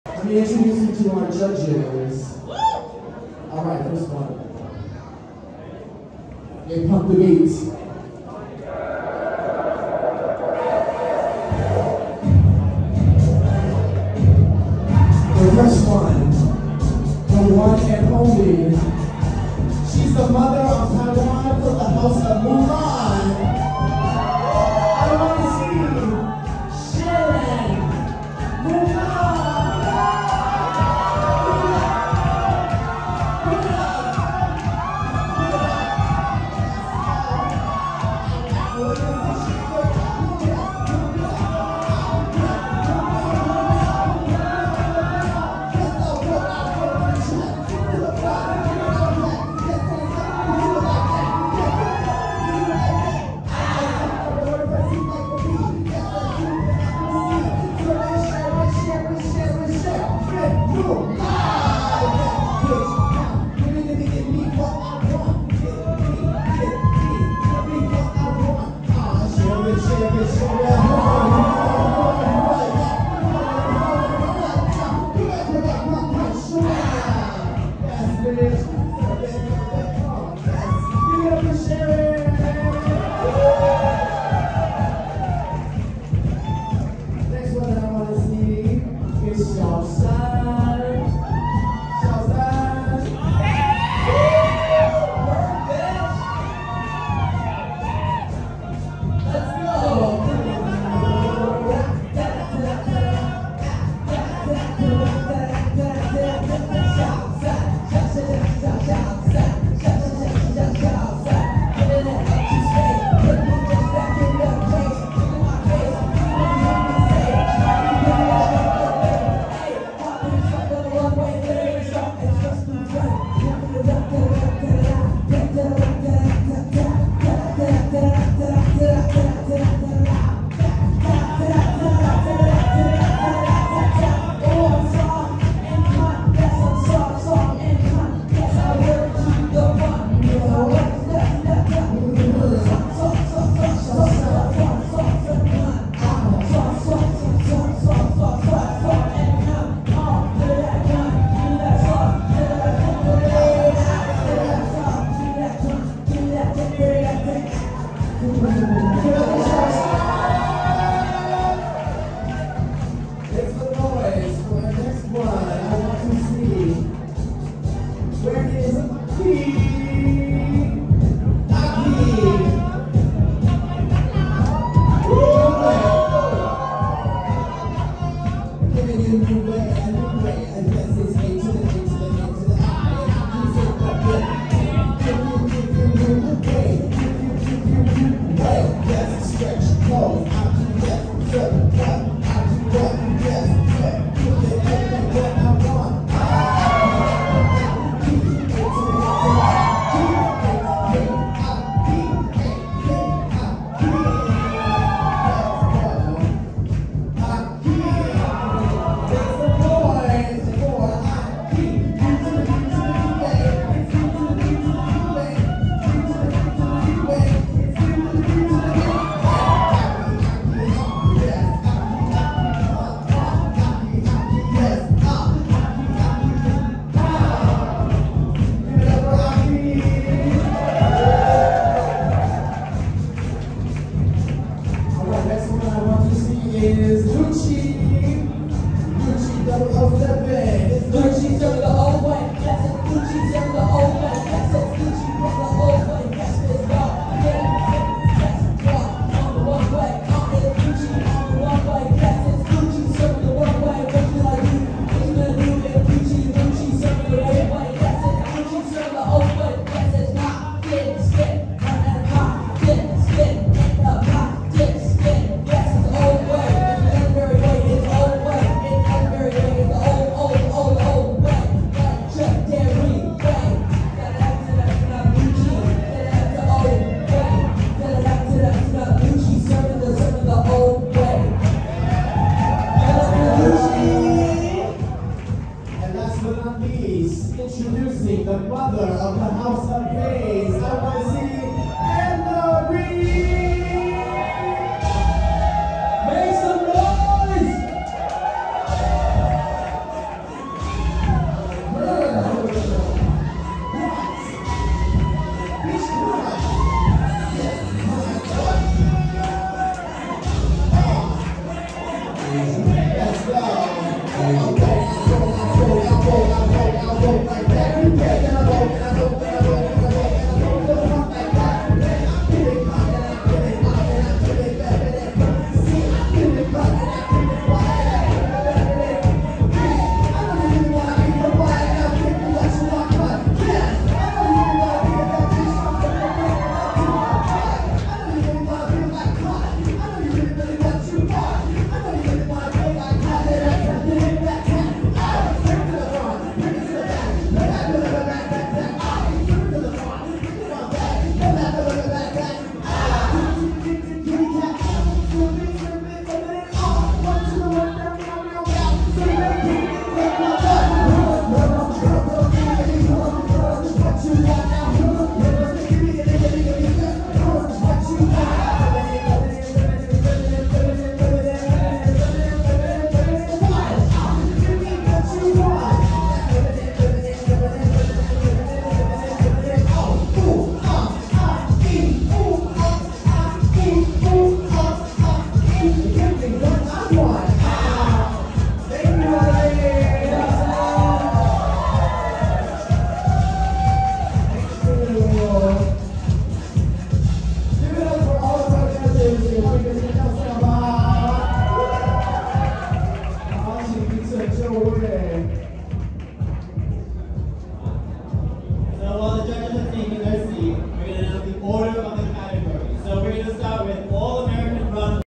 Judges LSS - The Walk of Art Kiki Ball - Nov. 20, 2022 Taipei
Featuring:
Sharron Mulan
Sann 007
Aki 007
Lucchi 007
Ellery Vase